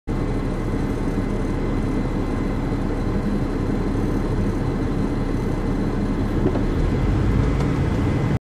Cyklist over for rødt på Strandboulevarden #2
Samme cyklist som på film #1 og #3